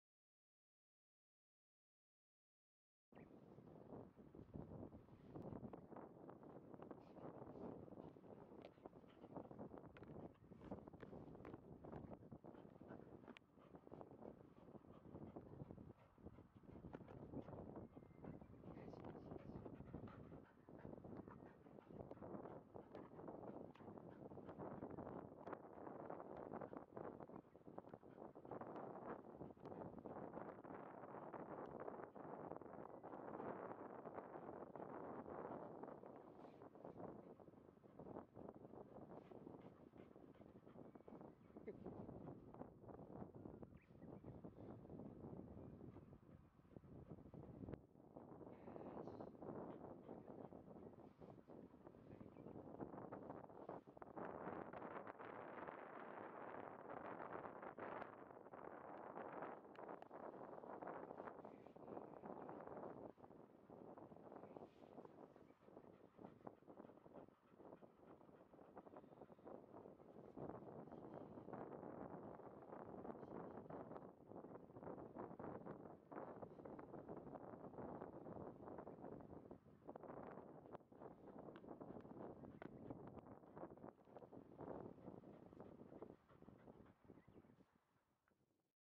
フリスビーの練習がおわったら・Labrador after disc training!
フィールド系ラブラドールレトリーバーです。
フリスビーの練習がおわったあとは、ひっぱりっこが定番です。
めちゃめちゃ風の強い日でした。
Labrador after disc training!
ヒーローディスクのHDX235というディスクを使っています。
穴があきにくく、長持ちするディスクで重宝しています。